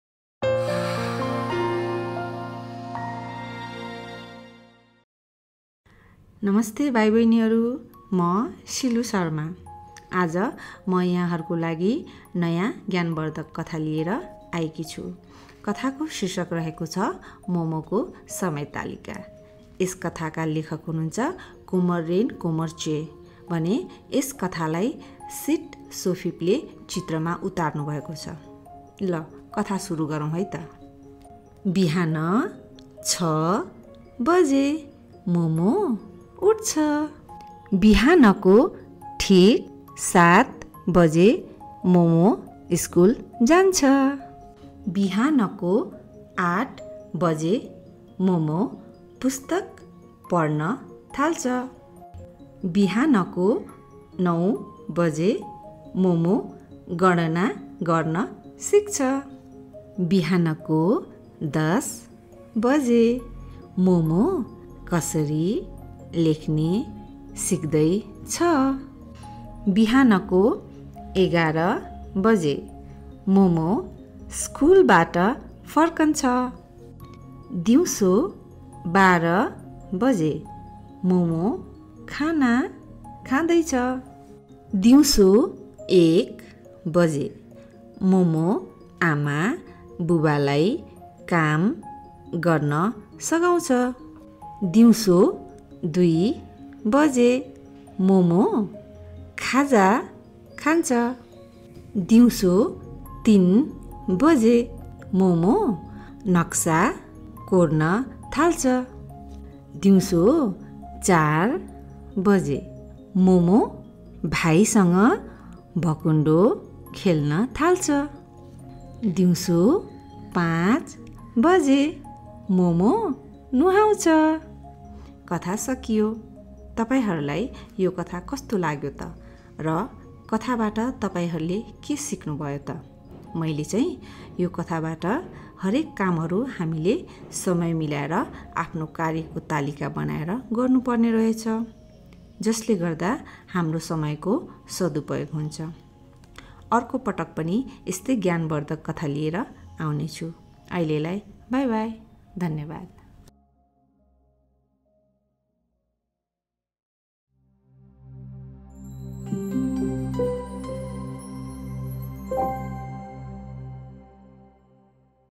Katha4Nepal || Shilu Sharma || मोमोको समय तालिका
Katha4Nepal is a social initiative to bring learning through stories for kids in Nepal and beyond.

मोमोले कसरी आफ्नो समय बिताउने गर्छ?

Author: Komar Rien Komar Cheh
Illustrator: Seat Sopheap
Publisher: The Asia Foundation - Let’s Read
Reading Level: Level 1